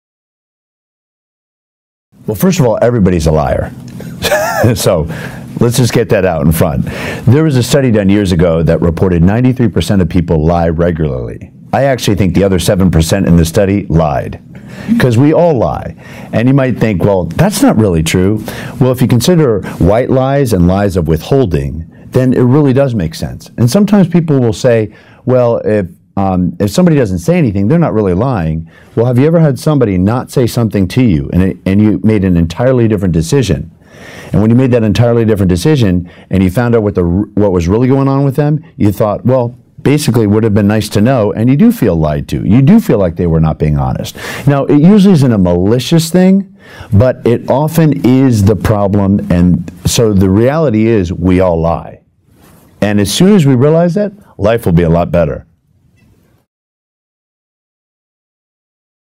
Everyone is a Liar
Don't be fooled - everyone is a liar! We all lie, and if you think this isn't true, you need to watch this!